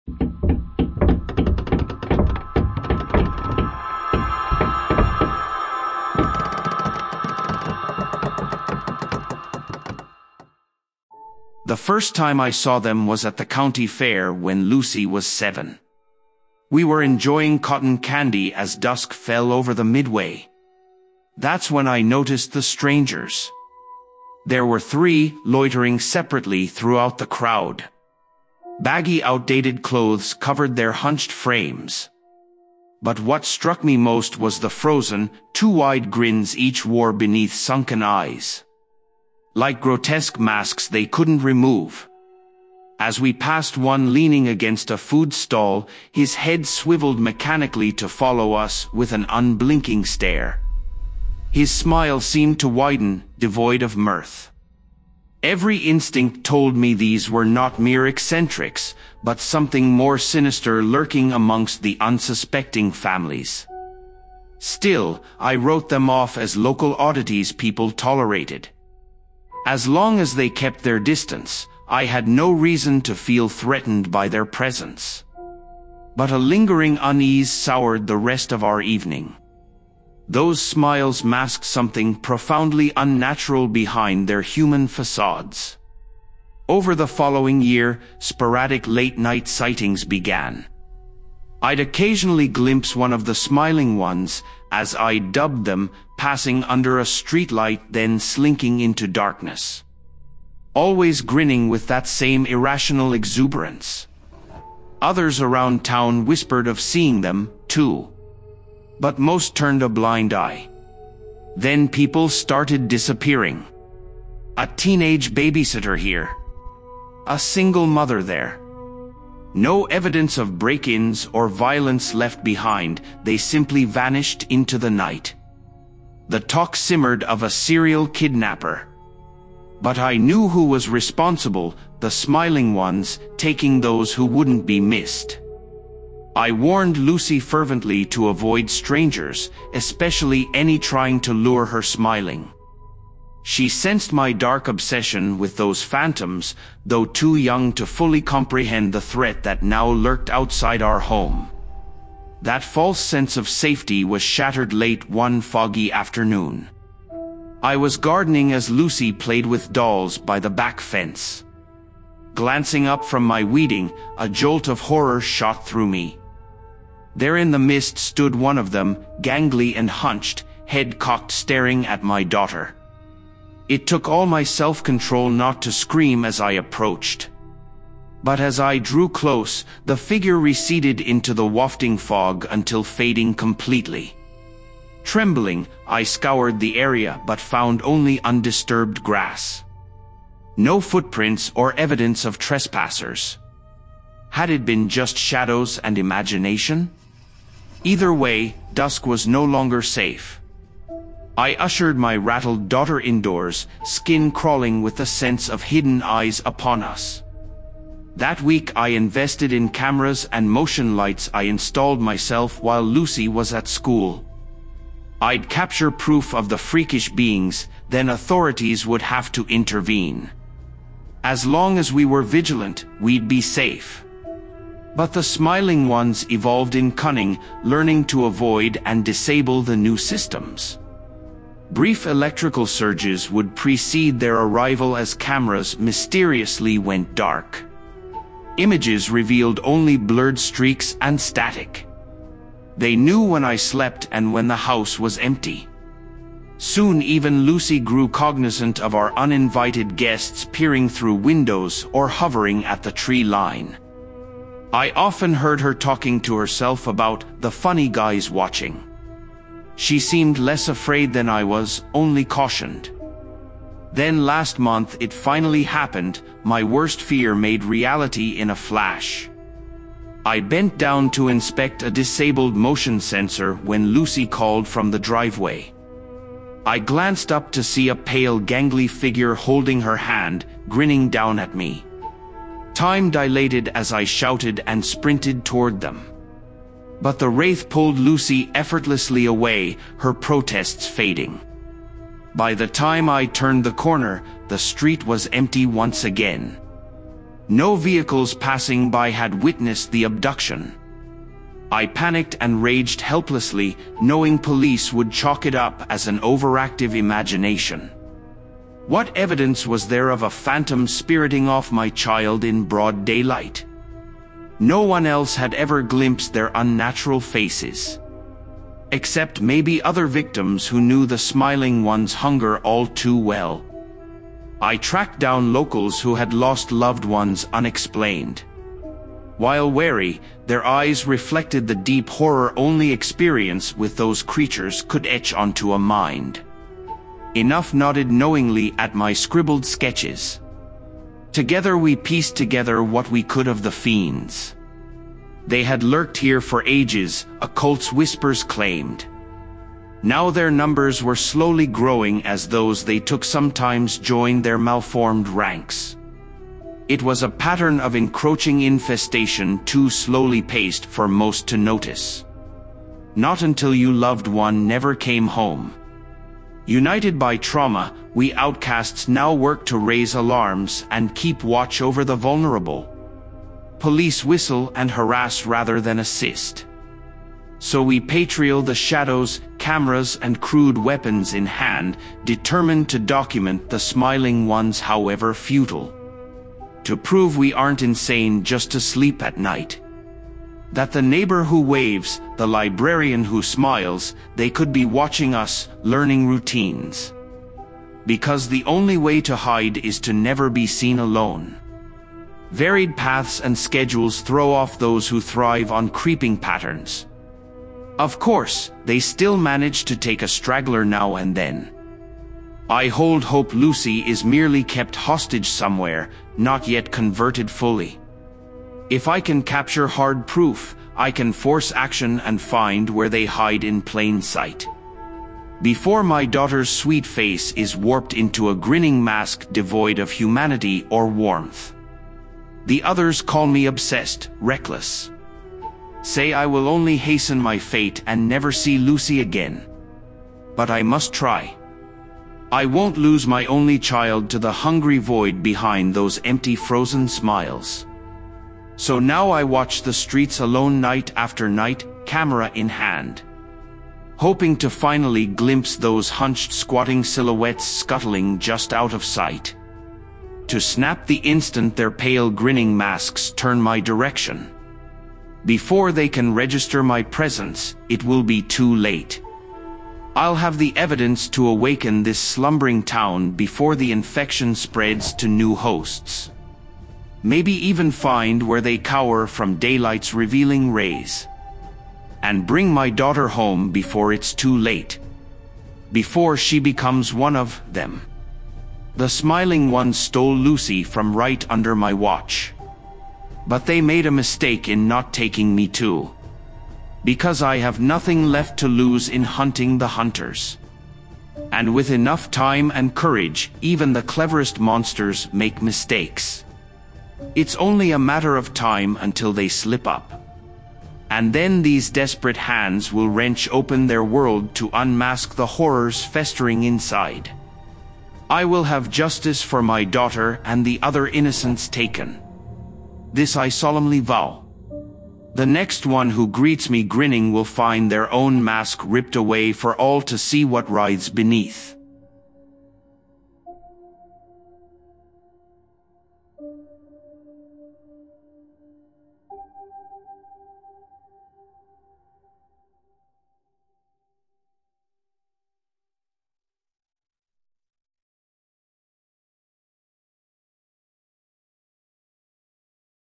We Are Being Hunted By the Smiling Ones | Creepypasta Stories Ep. 56
This creepypasta horror story follows a mother realizing her town is being infiltrated by unnerving strangers with frozen grins and an appetite for kidnapping residents. As her daughter is taken, she joins other victims determined to expose the sinister hidden agenda of these creatures called The Smiling Ones. But is a monster's mistake their only hope for revenge?
This original monster tale plays on fears of malicious entities hiding in plain sight as they secretly prey upon the vulnerable. The vivid depictions will give you chills as the tale builds toward a final showdown. When darkness wears familiar faces, who can you trust?